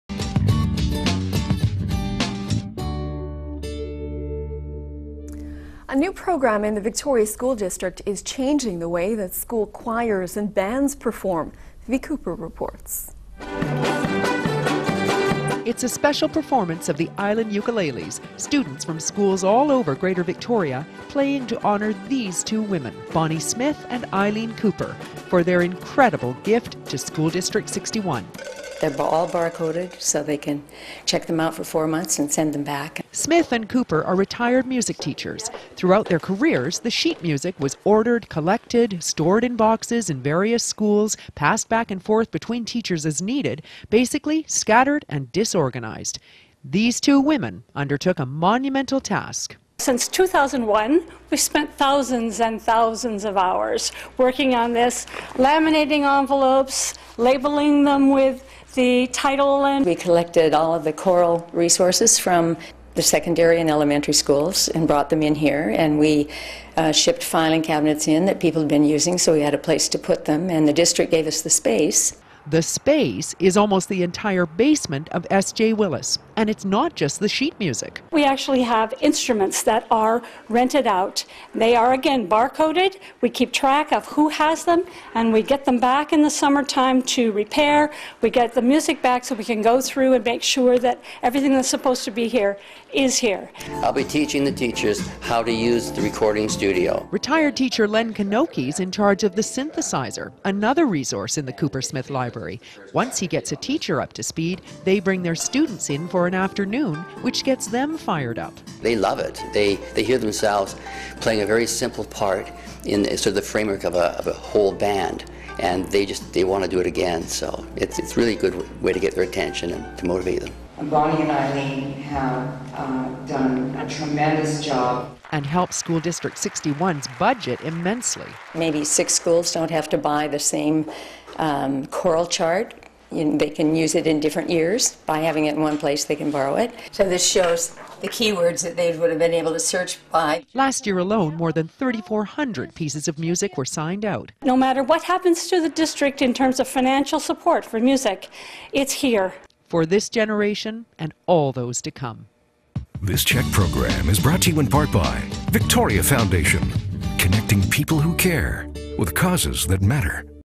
Vital People: Cooper Smith Music Library Collection
A new program in the Victoria School District is changing the way school choirs and bands perform. The Cooper Smith Music Library Collection is named after retired music teachers who have spent thousands of hours collecting and organizing sheet music, resources, and even musical instruments to lend out to the school district. The library translates into significant savings for SD61 as they can easily pool resources. No matter what happens to the district in terms of financial support for music, it's here for years to come. Last year alone more than 3,400 pieces of music were signed out.